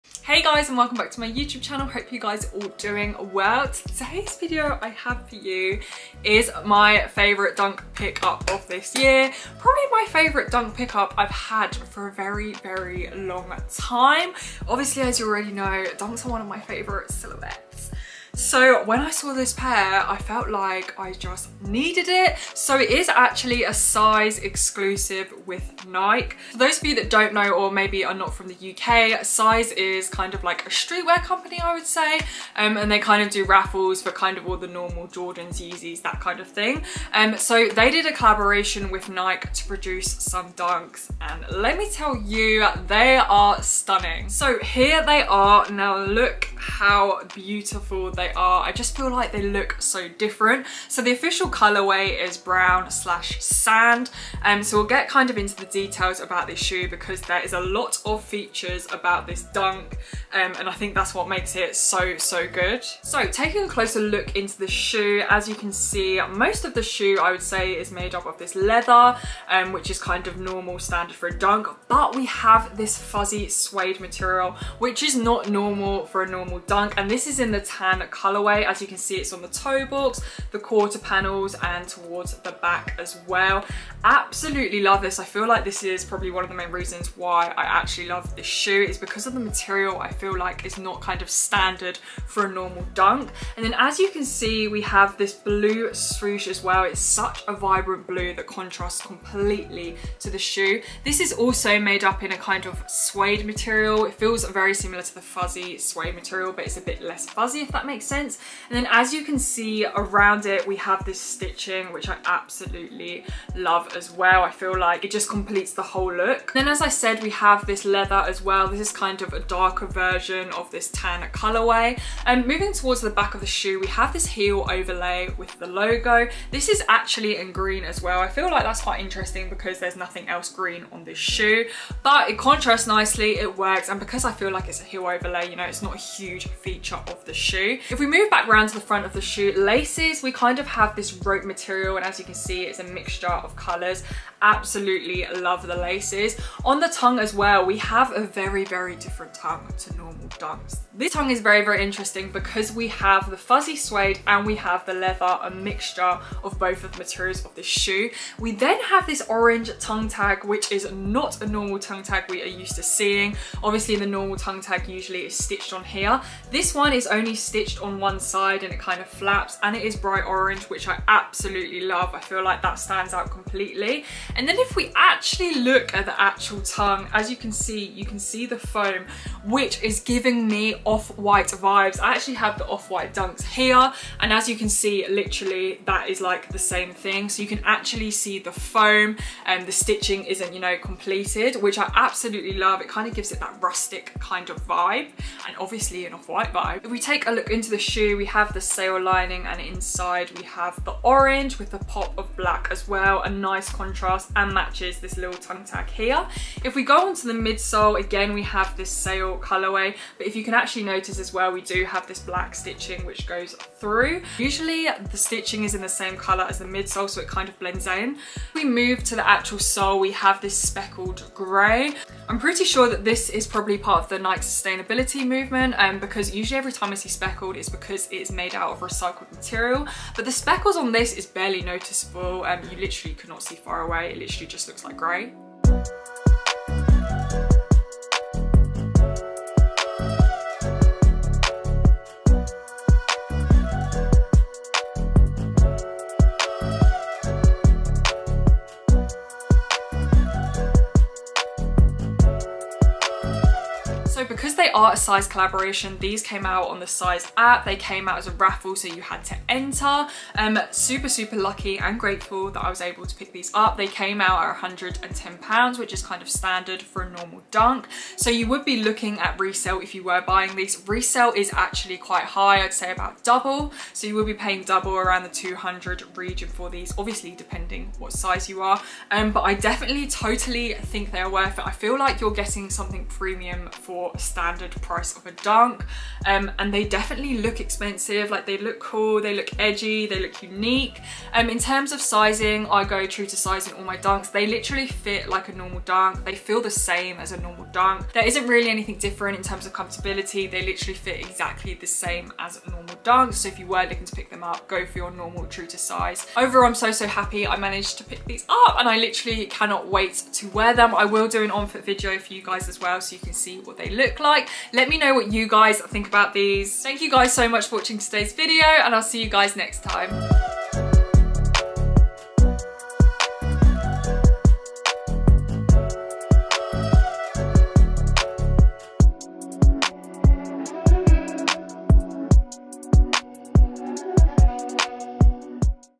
MUST HAVE! SIZE? X NIKE DUNK LOW
Suppperrrr excited about todays pick up! As you know I am a HUGE dunk fan & let me tell you these do not disappoint. Today's video is a review with an on feet - enjoy! xx

Nike Dunk Low size? Dark Driftwood